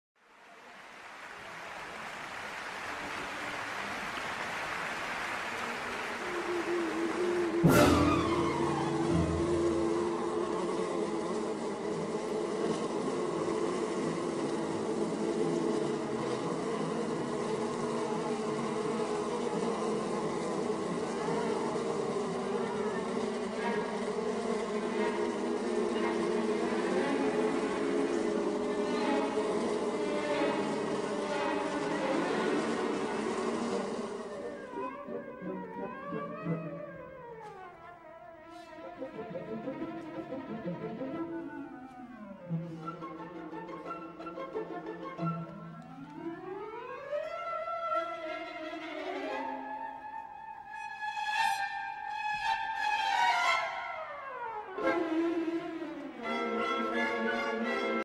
HoneyBee Medium
Checking on the hive of honeybees at  the COOK's COY GLEN Residence, Ithaca, NY.  Home owners had the pros come in and remove them correctly. Thank you...bee folks and the London Philharmonic. ese